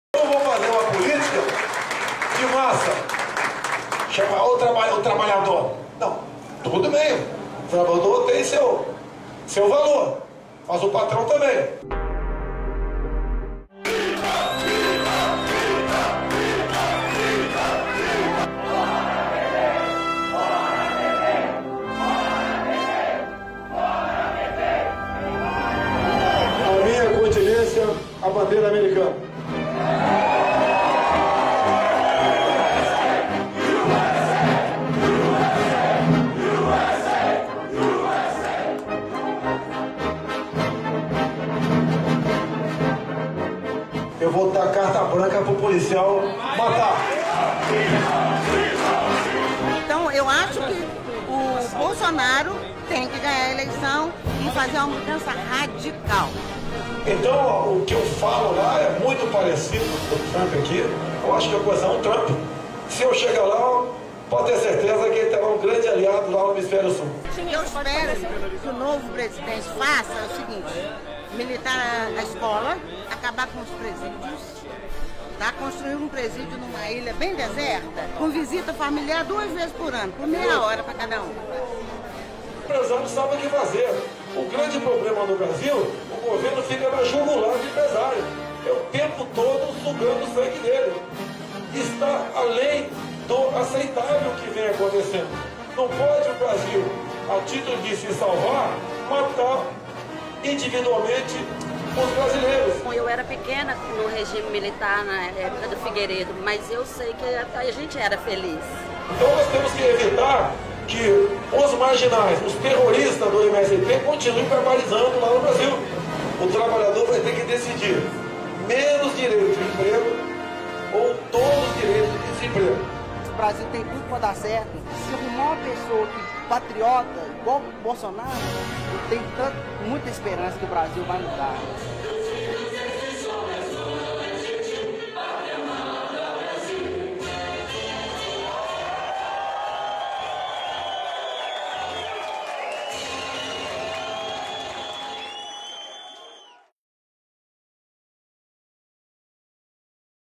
Tour Bolsonaro pelos EUA
A Tour Bolsonaro passa pelos EUA abrindo diálogo com elite financeira. Em busca de apoio, o pré-candidato fala de economia, apoia o fim da CLT, o avanço do agronegócio, mas sua audiência só pensa nas promessas que liberam os policiais para matar: